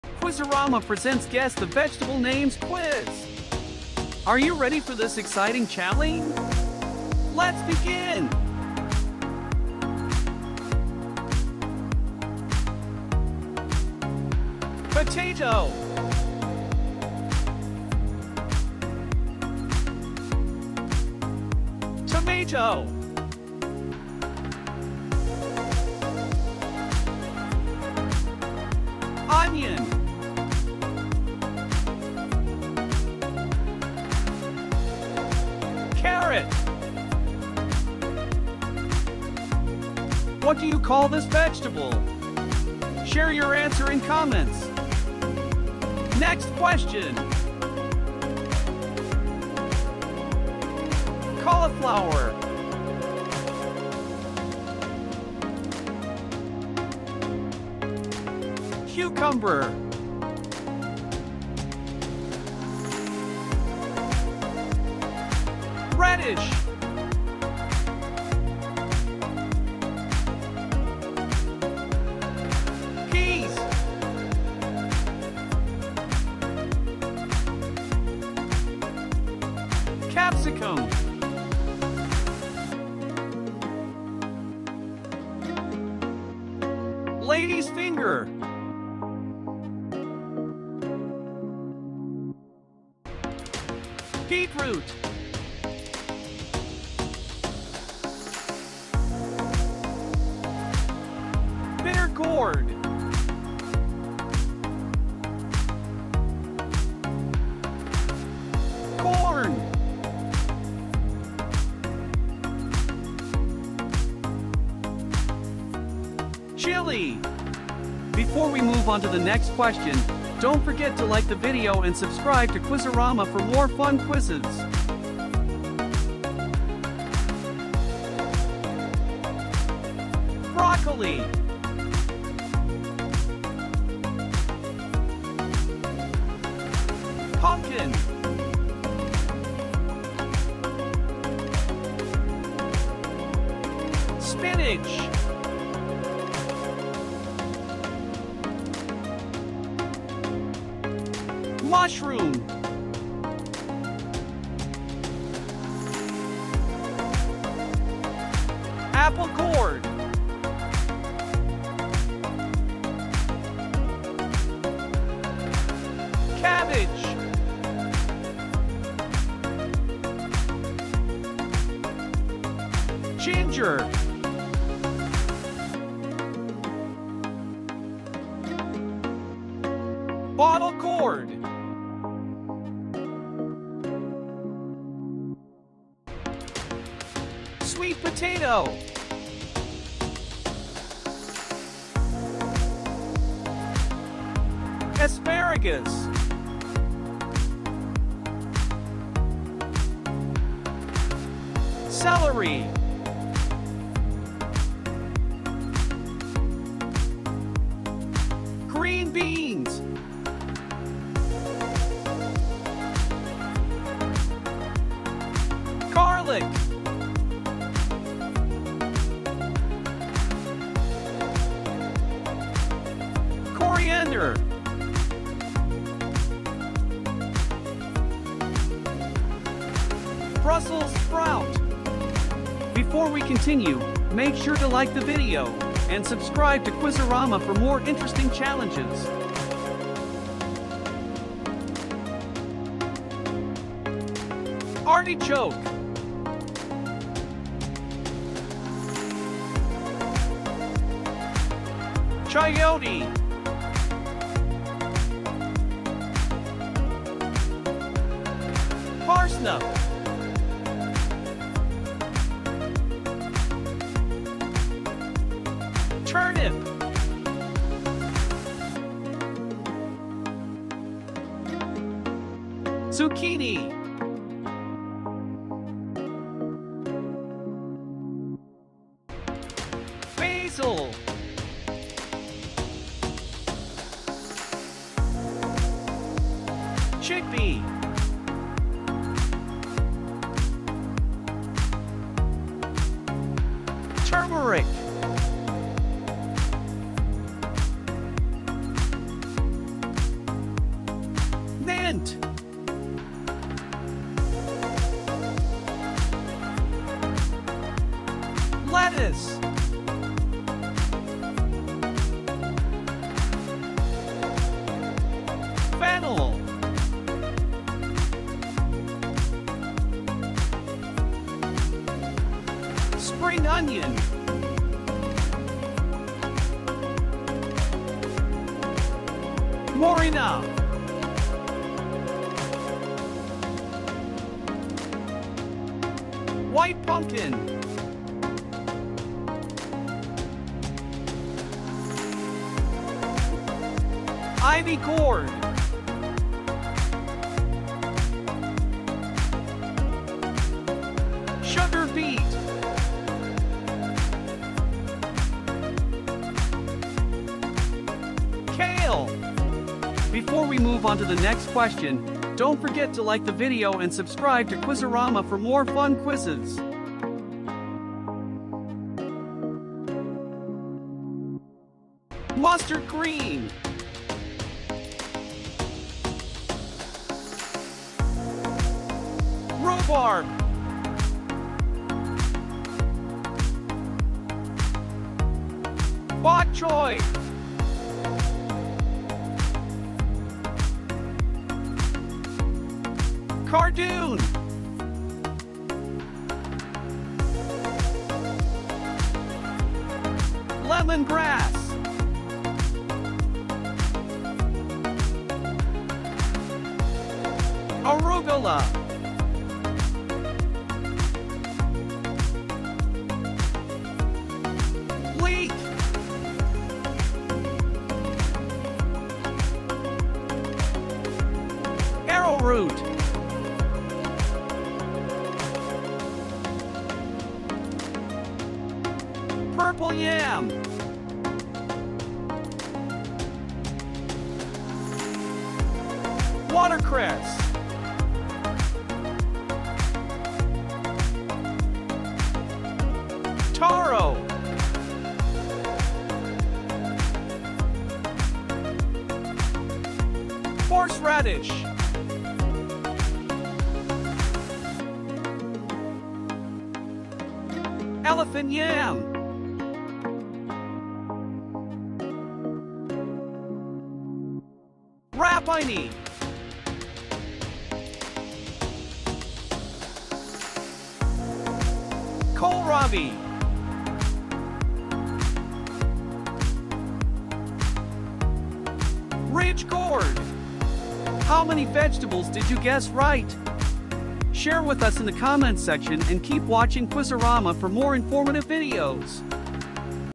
Guess the Vegetables | Fruit and Veg Quiz | Vegetable Quiz
This Guess the Vegetable Quiz will help you improve your vegetable vocabulary in English. This vegetable guessing game includes vegetable names with pictures and videos.
About : In this Guess the vegetables challenge you will find 63 different types of vegetables including
Tomato
Potato
Onion
Garlic
Carrot
Beetroot
Brinjal
Cauliflower
Cucumber
Reddish
Peas
Capsicum
Lady's Finger
Bitter Gourd
Corn
Chili
Broccoli
Pumpkin
Spinach
Mushroom
Apple Gourd
Cabbage
Ginger
Bottle Gourd
Sweet Potato
Asparagus
Celery
Green Beans
Garlic
Coriander
Brussels Sprout
Artichoke
Chayote
Parsnip
Turnip
Zucchini
Basil
Chickpea
Turmeric
Mint
Lettuce
Fennel
Spring Onion
Moringa
White Pumpkin
Ivy Gourd
Sugar beet
Kale
Mustard Green
Rhubarb
Bok Choy
Cardoon
Lemon Grass
Arugula
Leek
Arrowroot
Purple Yam
Watercress
Taro
Horseradish
Elephant Yam
Rapini
Kohlrabi
RidgeGourd
So, are you ready to guess vegetable names?